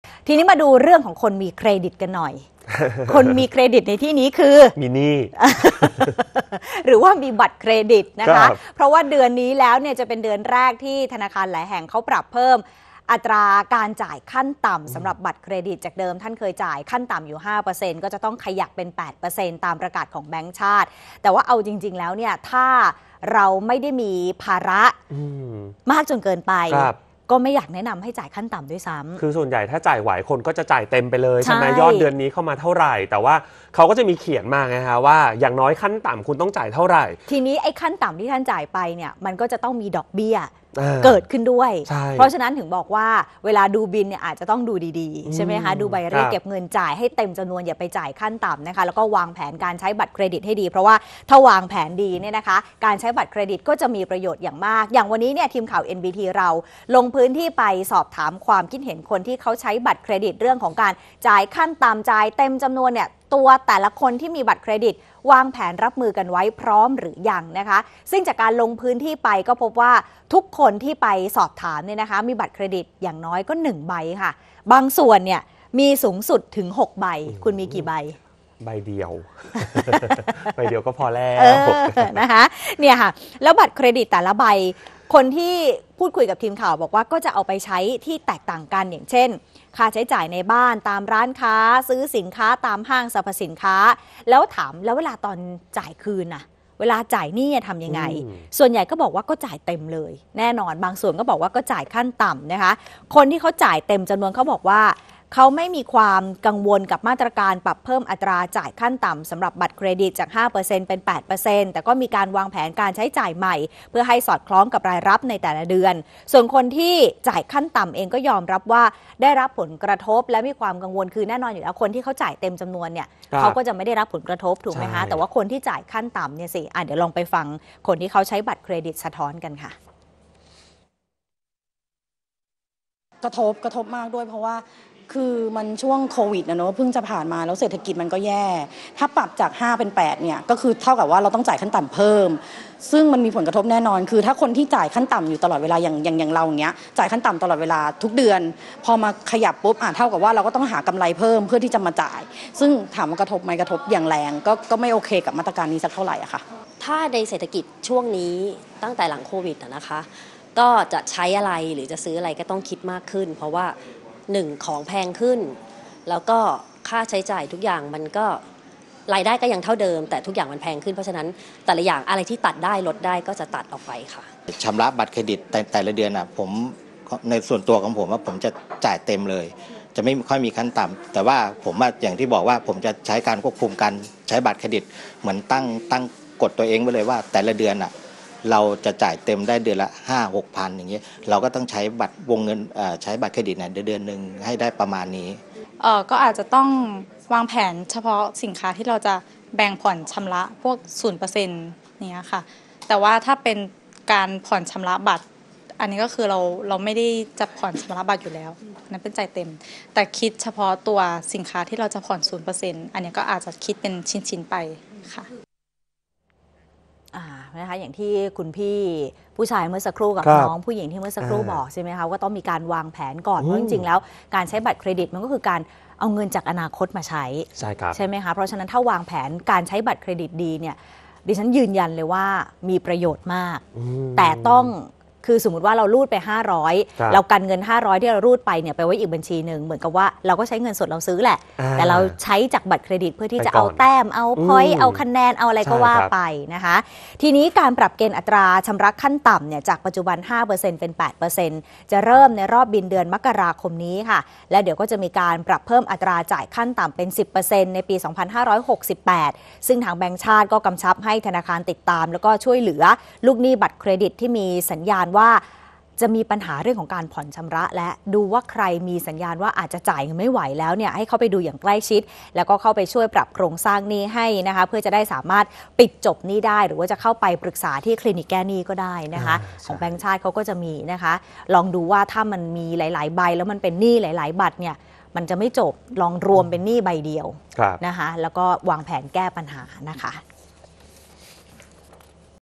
ผู้ใช้บัตรเครดิตเตรียมรับมือจ่ายค่างวดเพิ่มขึ้น ข่าวค่ำ วันที่ 3 มกราคม 2567
เดือนนี้ จะเป็นเดือนแรกที่ธนาคารหลายแห่ง จะปรับเพิ่มอัตราจ่ายขั้นต่ำ สำหรับบัตรเครดิต จากเดิม 5% เป็น 8% ตามประกาศของธนาคารแห่งประเทศไทย ทีมข่าว NBT จึงลงพื้นที่สอบถามความคิดเห็นผู้ใช้บัตรเครดิต เบื้องต้นมีการวางแผนรับมือไว้แล้ว